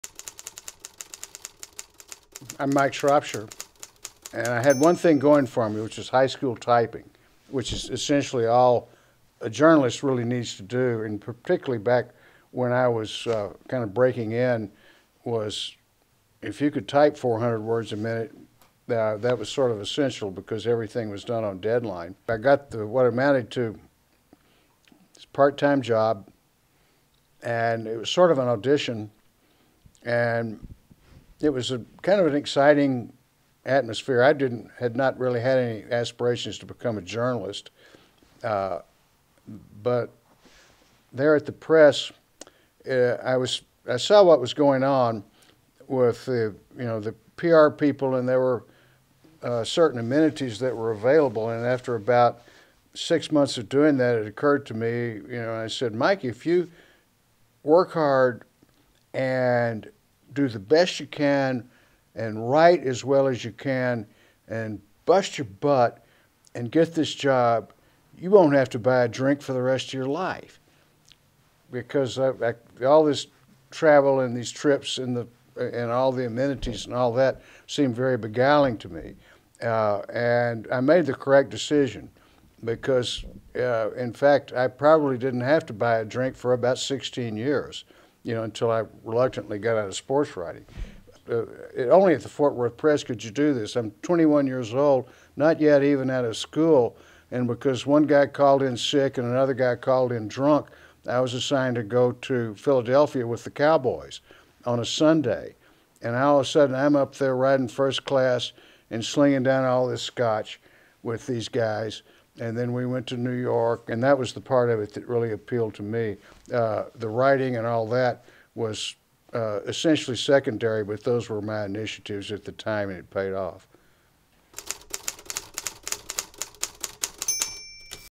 The Sports Page: Interviews Mike Shropshire Part 1
Part 1 of a candid interview with sports journalist/author, Mike Shropshire
Produced/Directed by: Adam Dietrich
Director of Photography/Editor: Elliott Gilbert, II